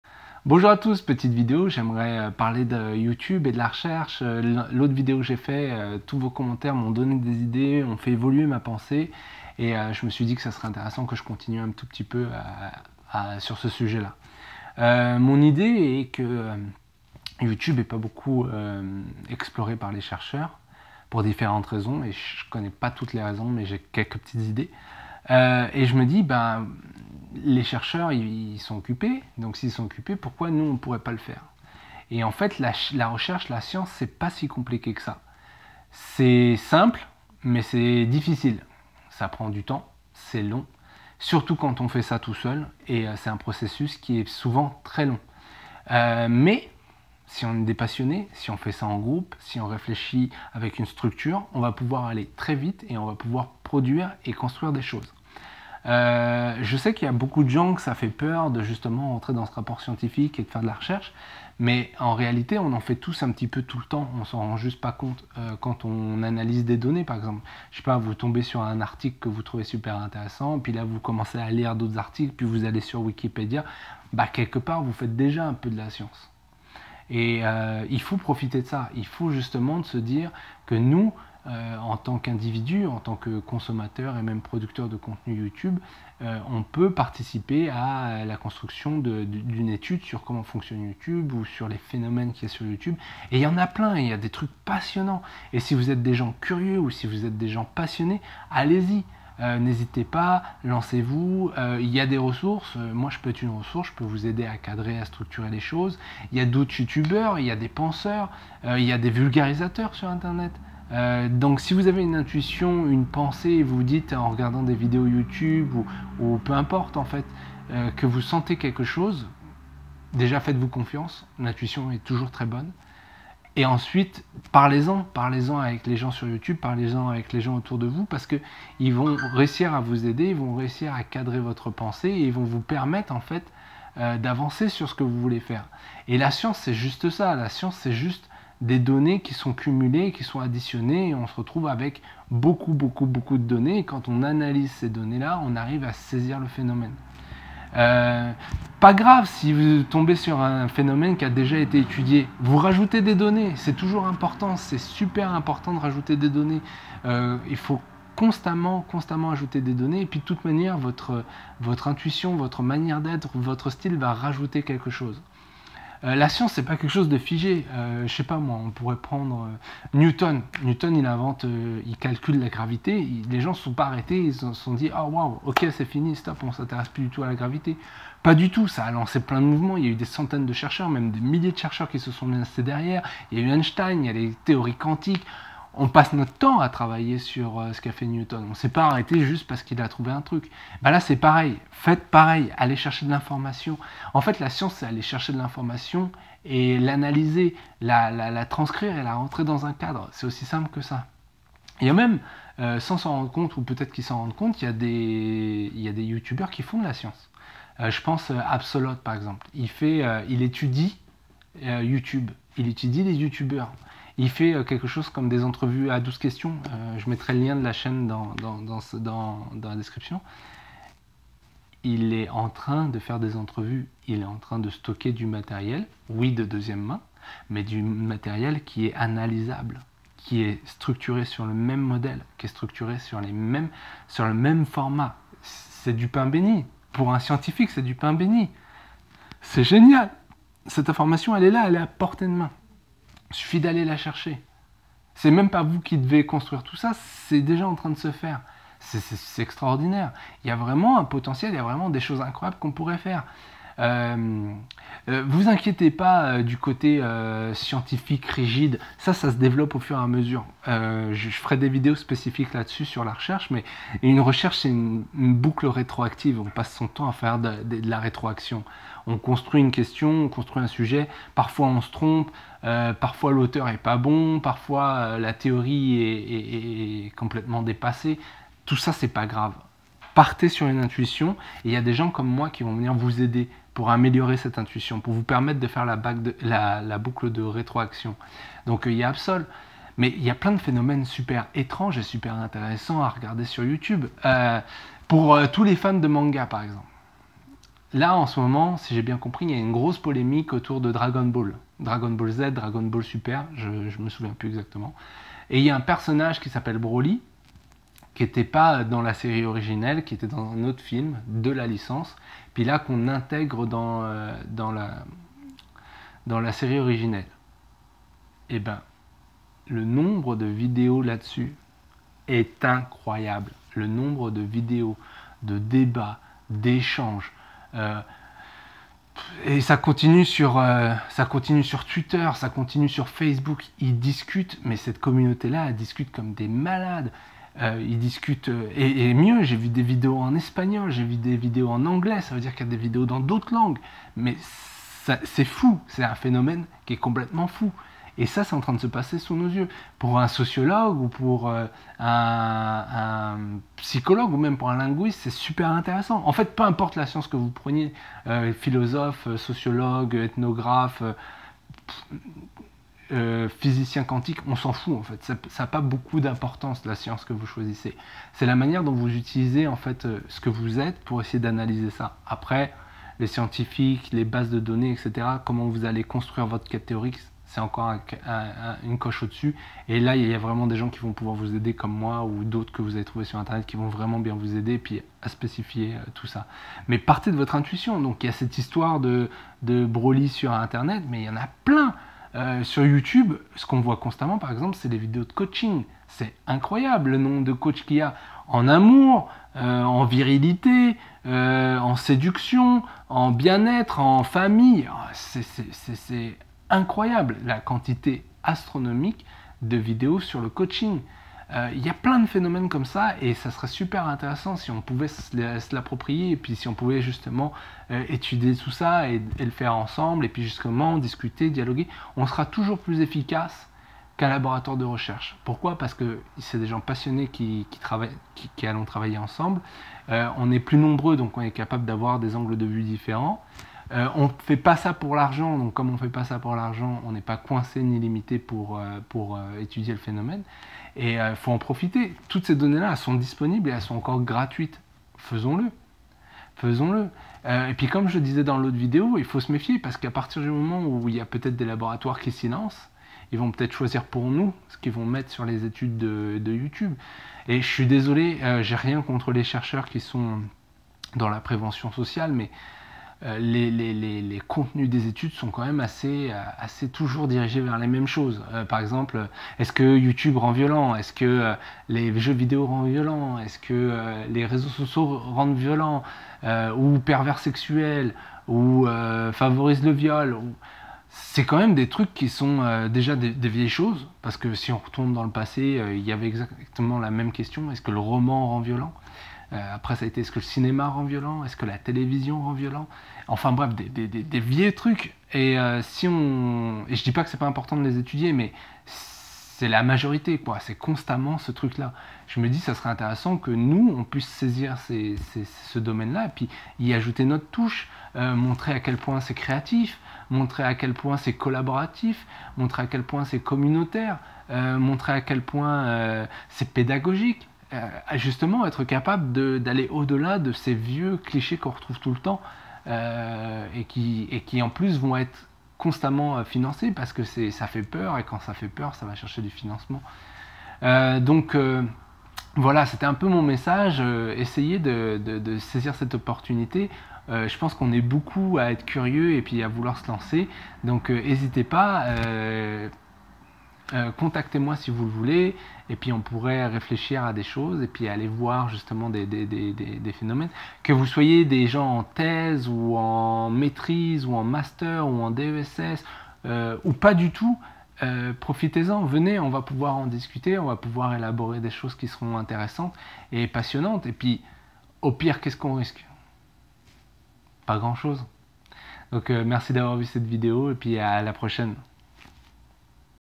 Science, passion et YOUTUBE !
Et si on faisait de la recherche scientifique ensemble sur Youtube ? Afin de comprendre les phénomènes sociaux et psychologiques qui composent les comportements sur ce réseaux sociales. Il faut en profiter ce terrain de recherche est encore vierge.

Merci